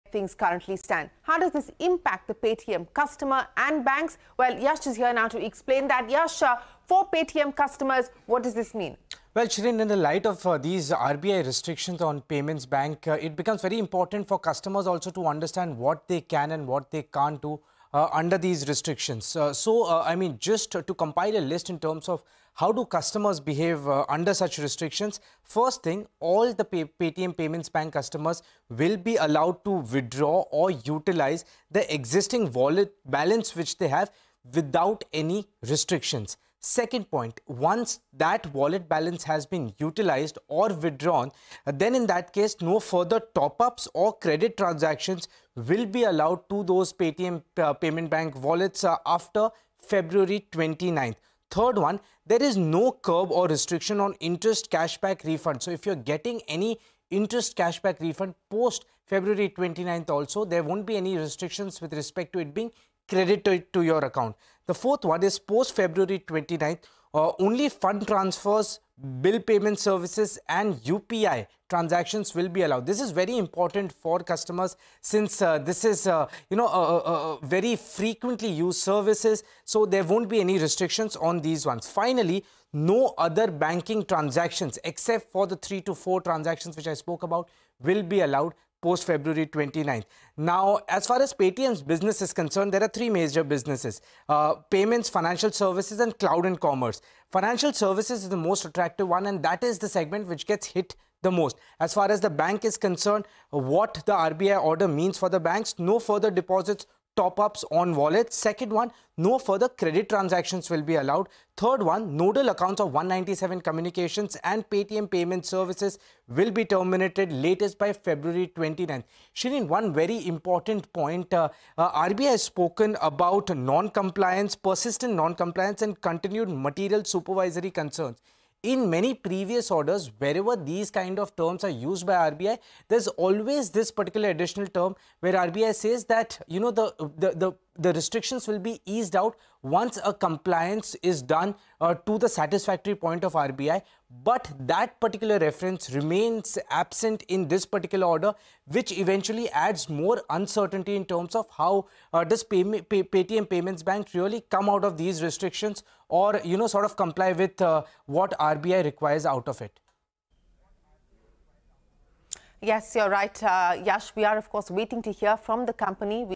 Explained: Impact Of RBI Curbs On Paytm Payments Bank Customers And Banks | CNBC TV18
What do the RBI restrictions on Paytm mean for customers & banks? Yash Jain explains.

n18oc_business